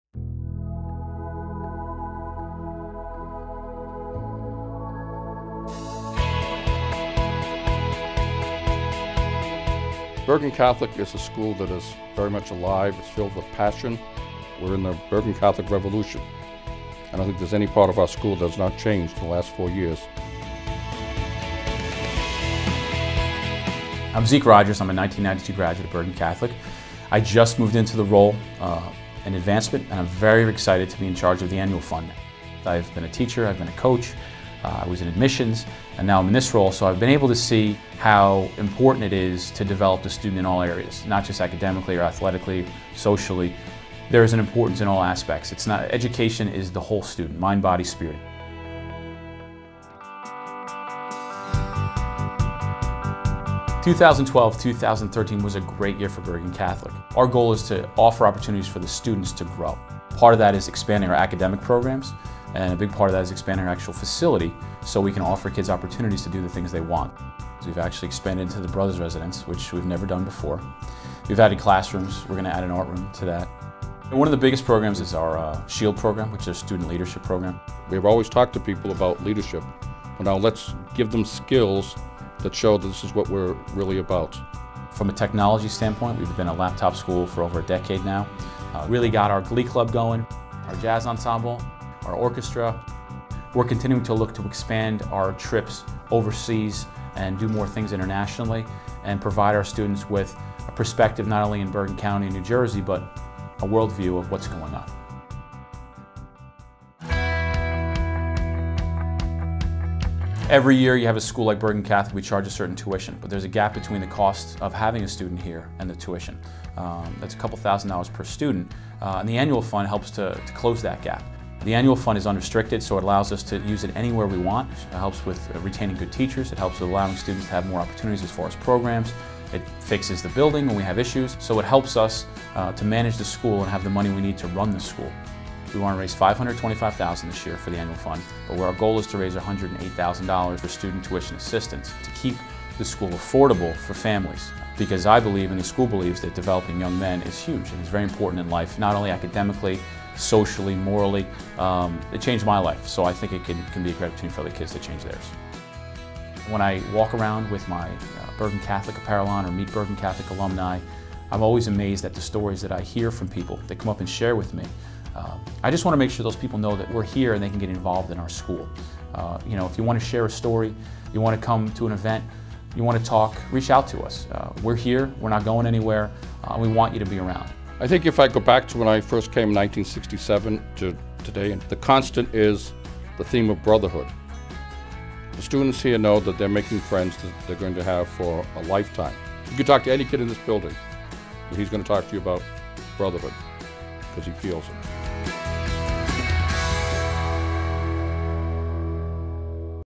Bergen Catholic High School Fall 2013 Update
The Crusader Annual fund is a cornerstone effort that enables us to fulfill the mission of Bergen Catholic and develop the next generation of leaders. This video provides a recap of where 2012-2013 funds were invested and a peak into the future plans to continue fulfilling the mission.